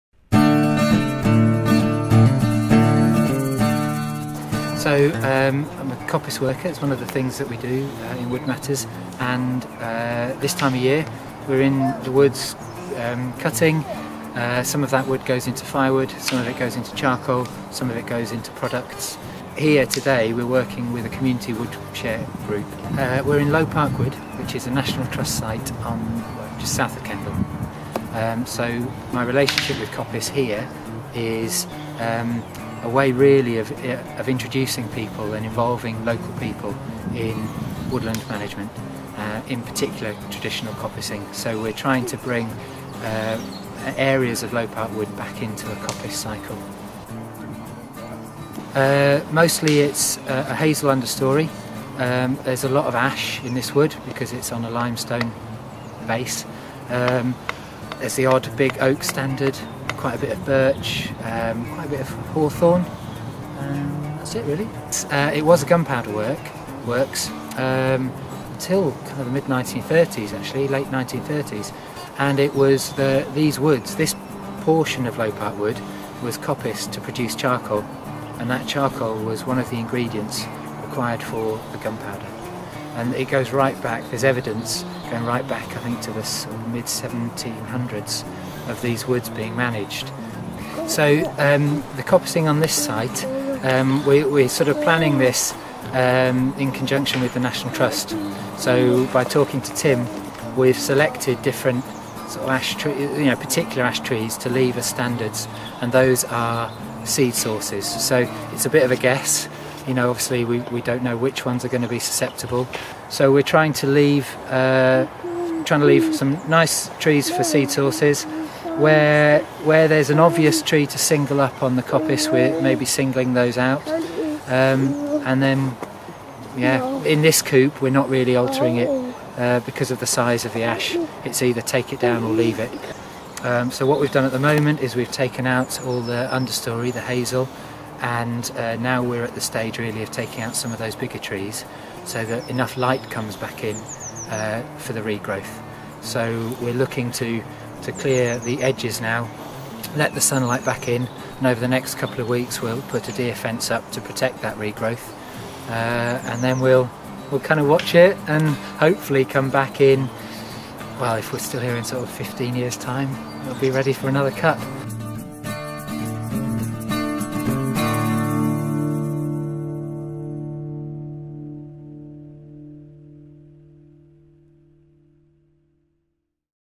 Community Coppice at Sizergh
First Published 2018---

Gareth Thomas from Woodmatters talks us through the coppicing process and explains the community wood share project which introduces local people to woodland management.